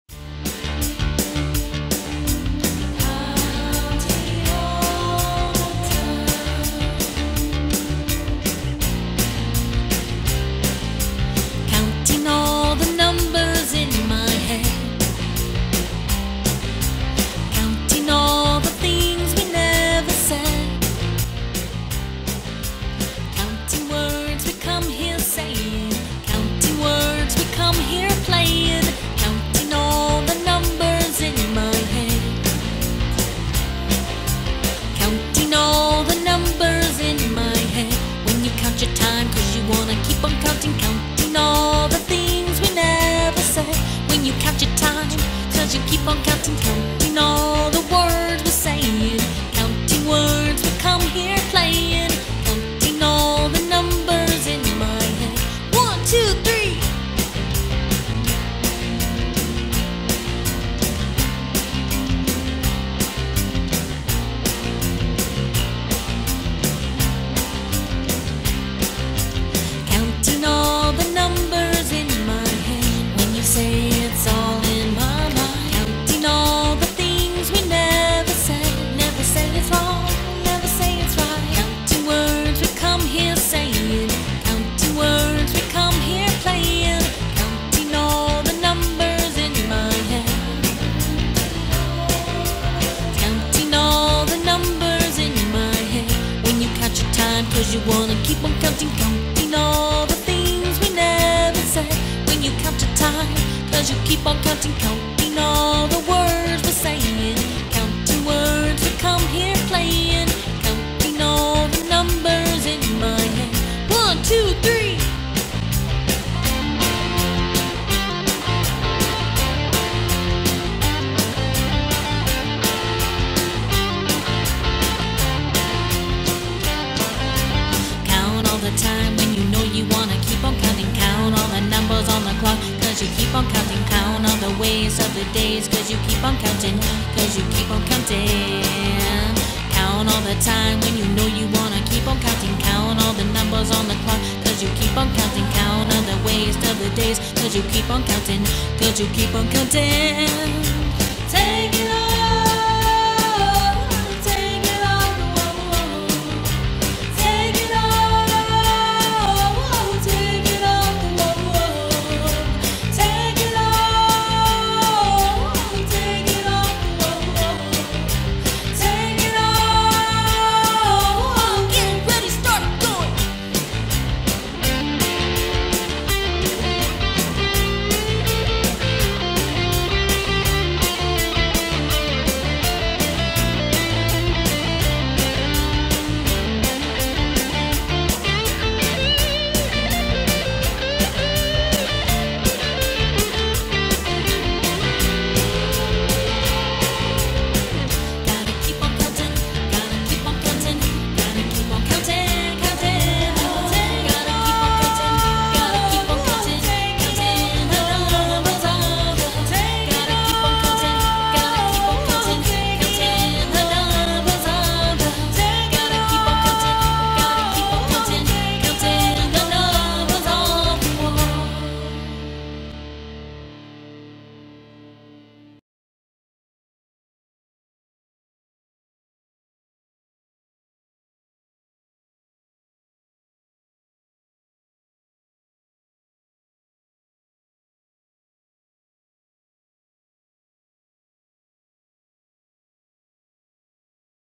numbers mirror image Video
this video is a video of counting all the numbers in my head and for people who want to count all the numbers all the time do you count all the numbers you keep thinking about